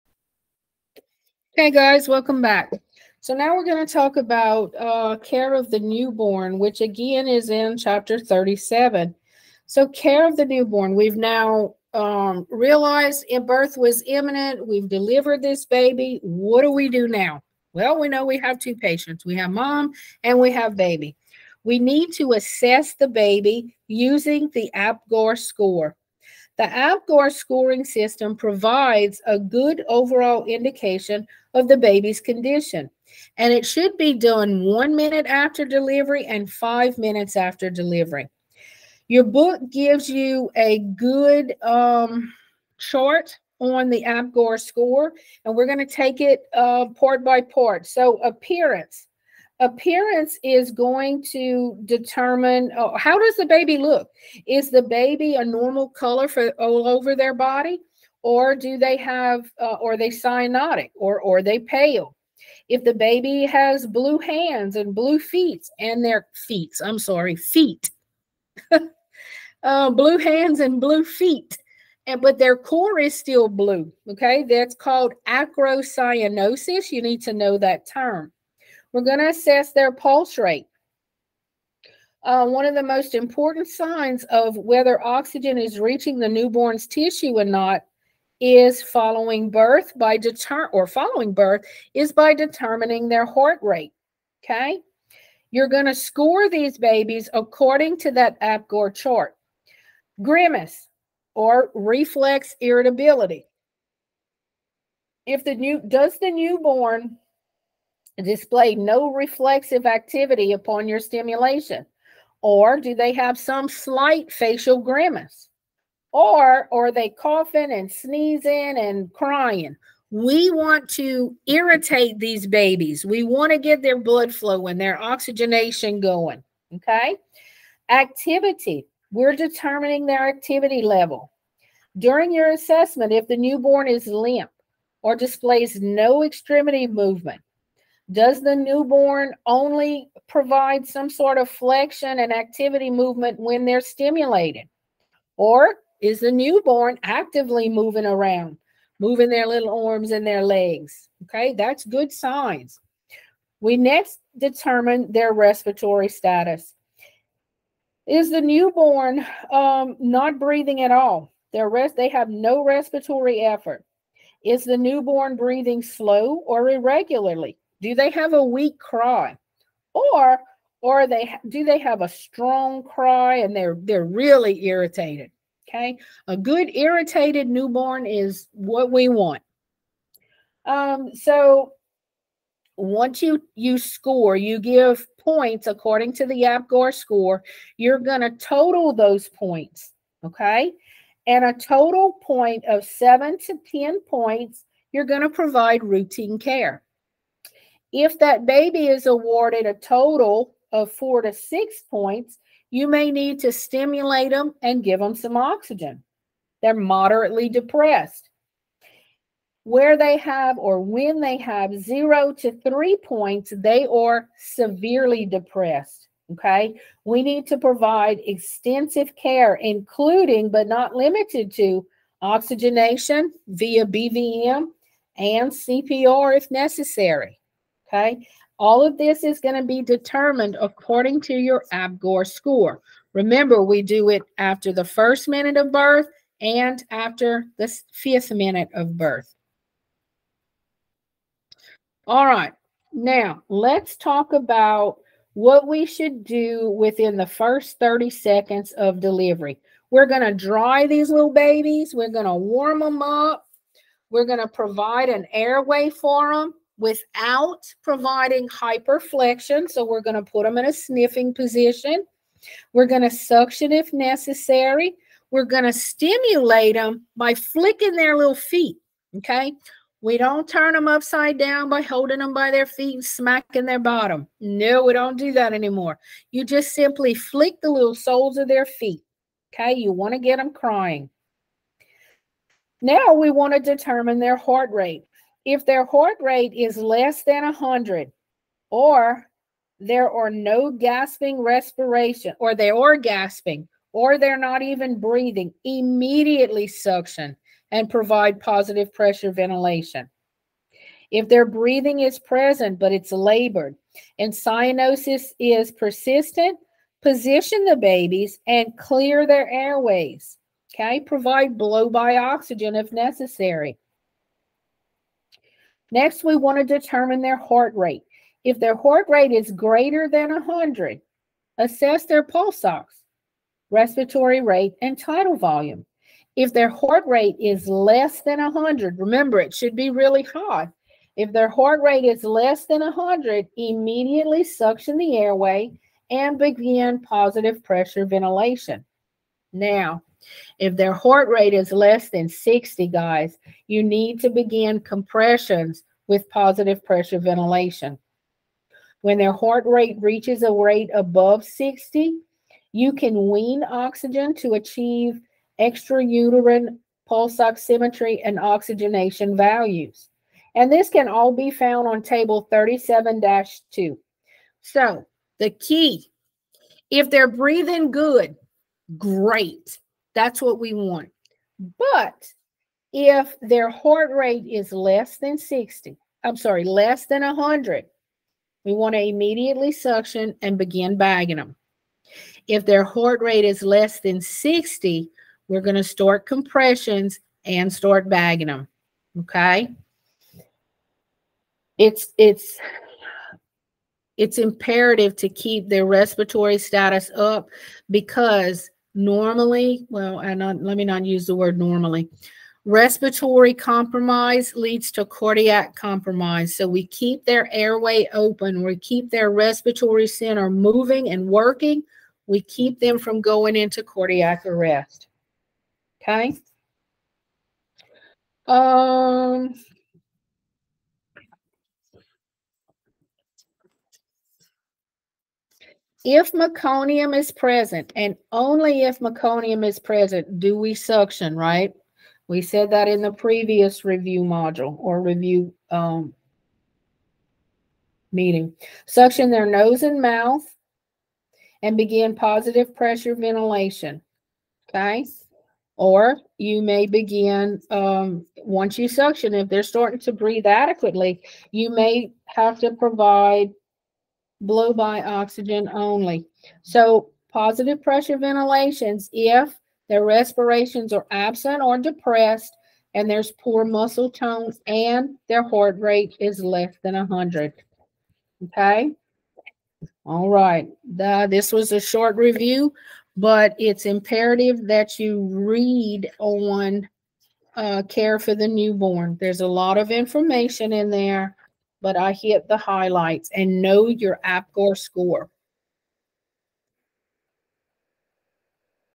Module 5 Review Video - Part 3 - (Ch 9 & 37-39)
This is a module review video for Chapter 9 & 37-39 of the Pearson Prehospital Emergency Care 12th Edition book.